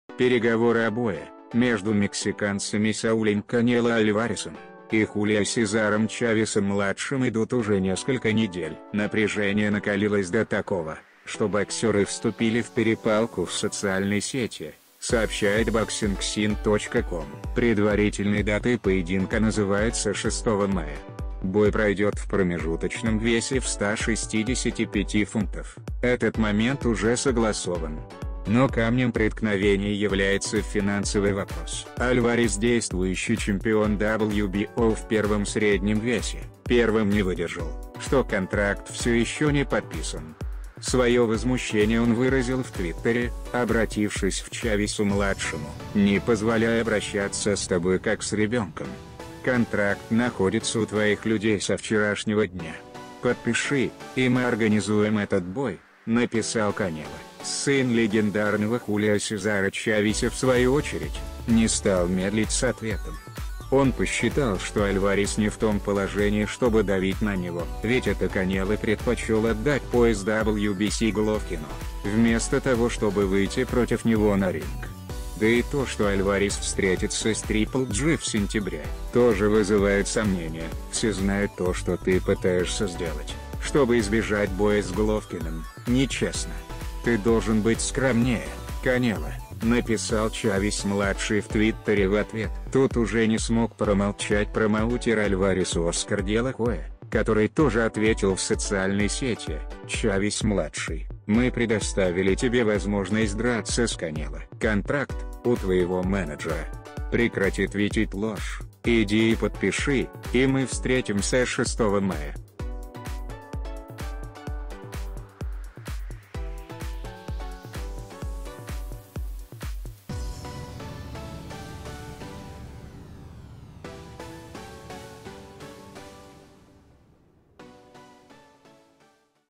Чавес: "Канело, Избегая Головкина, ты должен быть скромнее"
"Избегая Головкина, ты должен быть скромнее, "Канело". Как Чавес и Альварес повздорили в Сети. Мексиканские боксеры никак не подпишут контракт на бой.